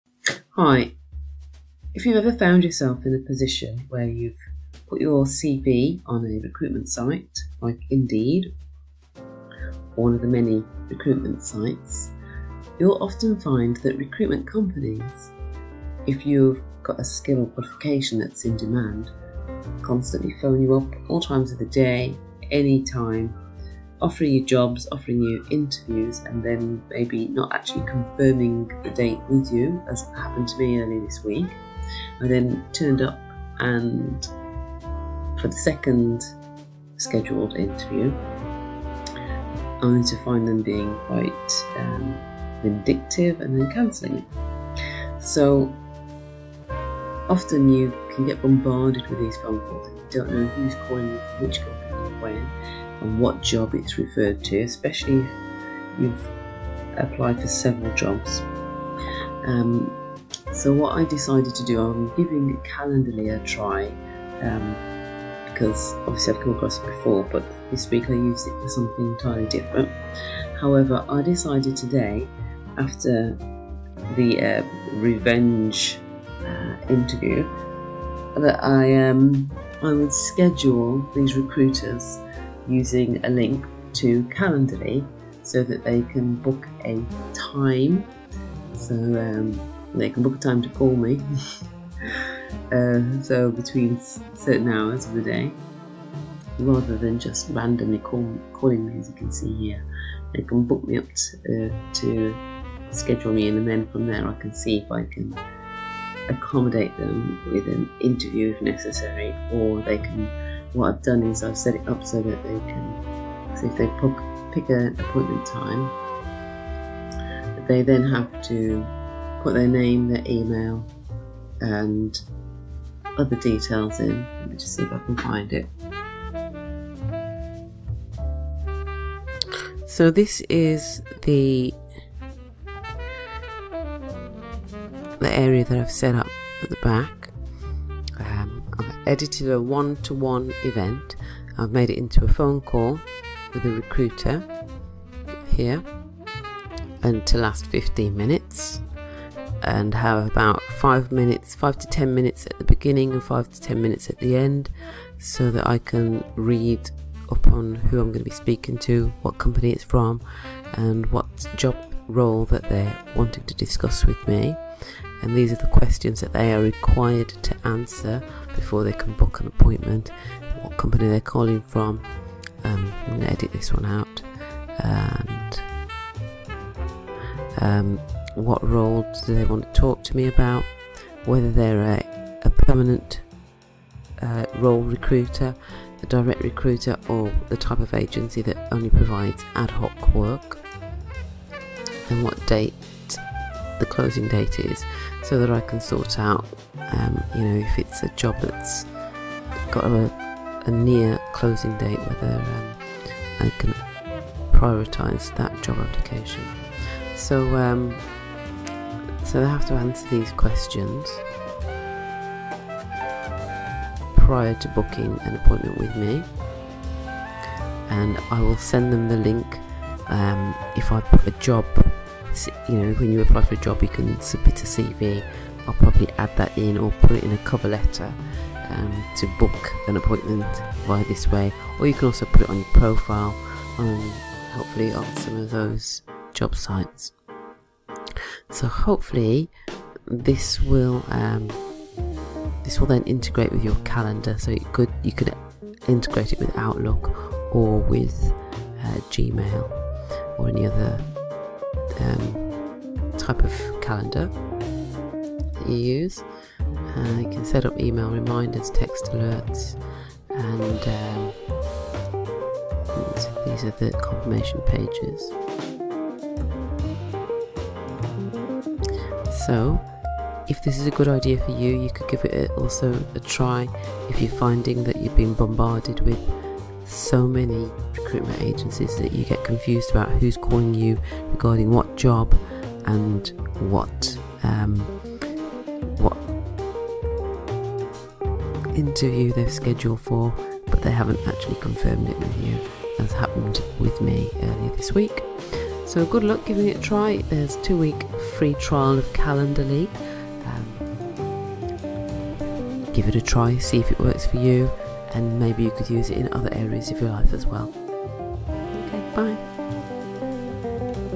How to manage Recruitment Agencies on Indeed using Calenderly
Use Calenderly to schedule phone calls and speculative calls from recruiters who randomly call you for various job roles on Indeed or many other job sites like CV Library, Reed or Glassdoor. Have them give you details of the job before the call so that you can decide if you want to take the call. Calenderly offer you a 14 day trial, which can also be used by businesses, life coaches and small businesses to help manage their time. It integrates with your website and can also be used with other Calenders.